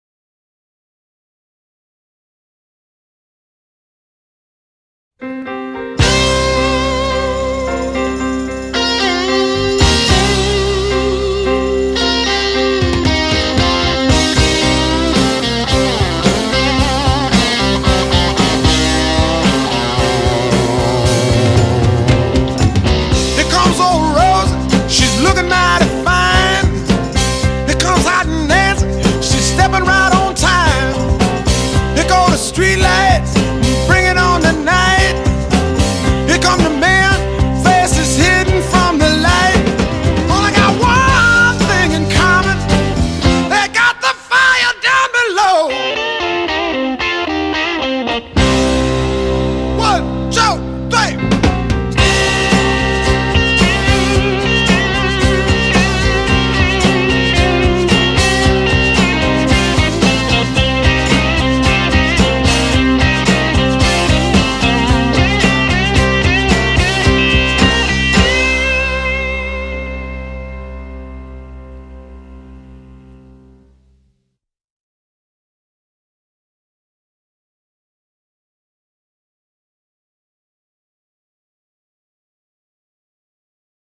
Old Rosie
Motorsailor Old Rosie underway with Hot Nancy and The Man